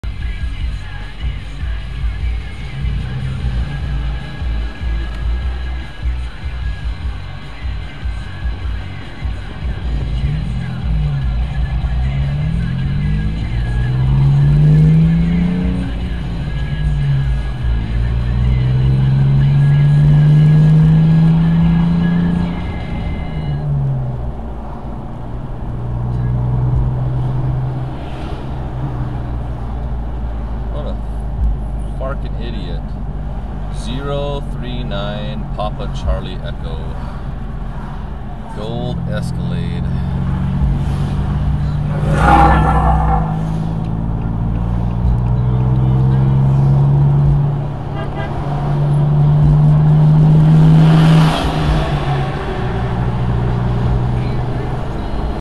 No so patient Caddy driver
Really did not want to be behind the large truck. In the end he was.